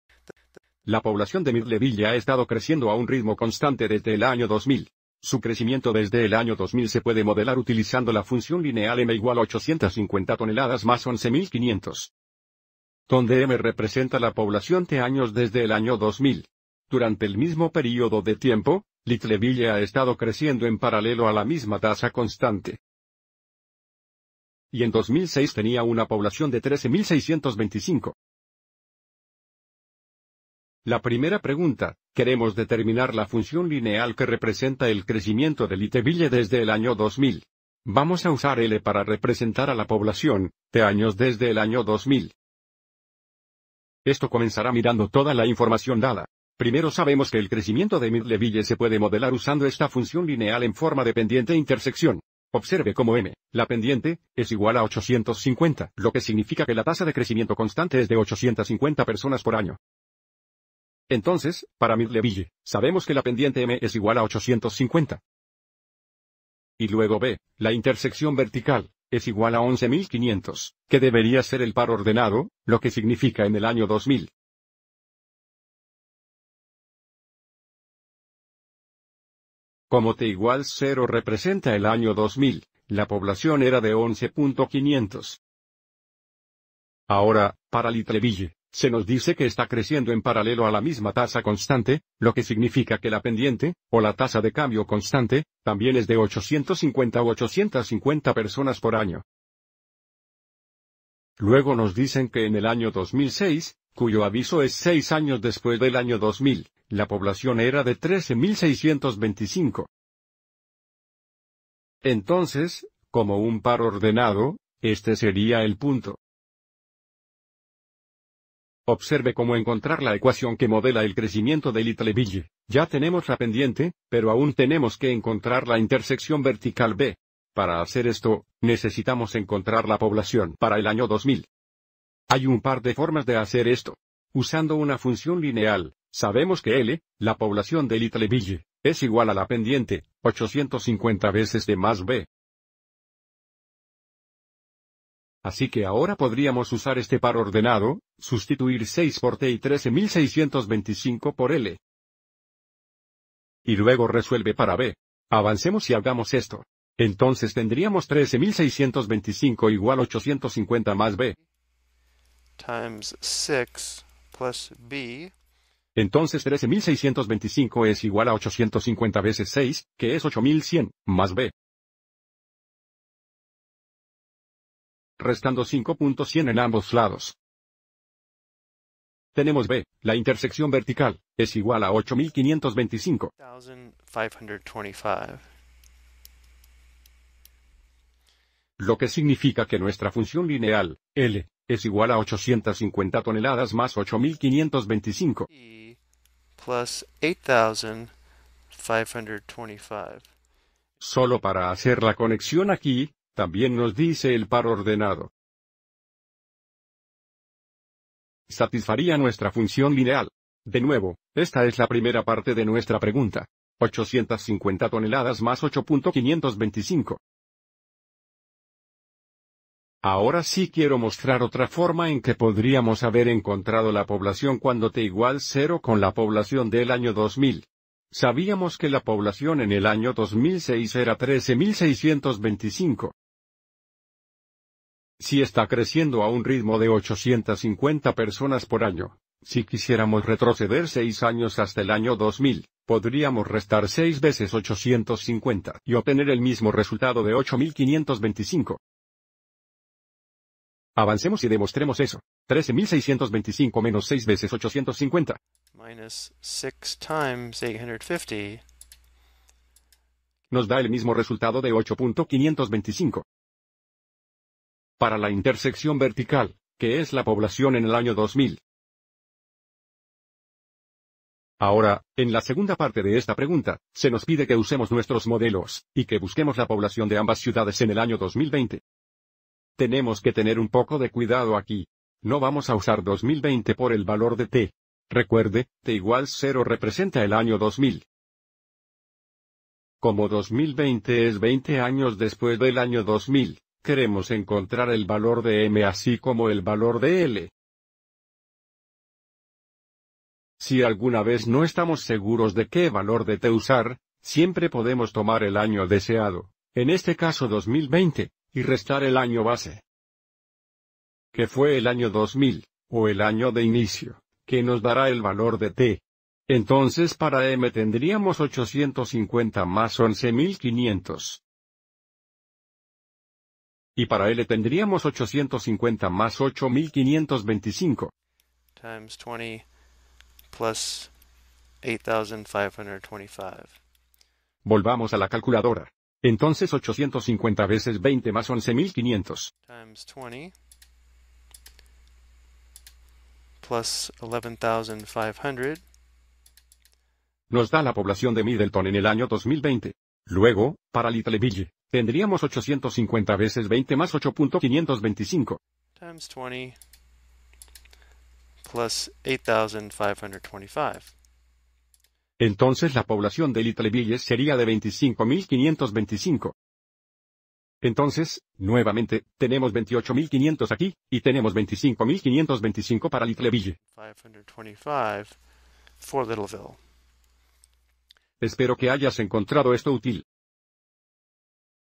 Ej: Crecimiento poblacional de función lineal (aplicación de líneas paralelas)
Este video explica cómo determinar una función lineal que modela el crecimiento de la población que es igual a otra ciudad.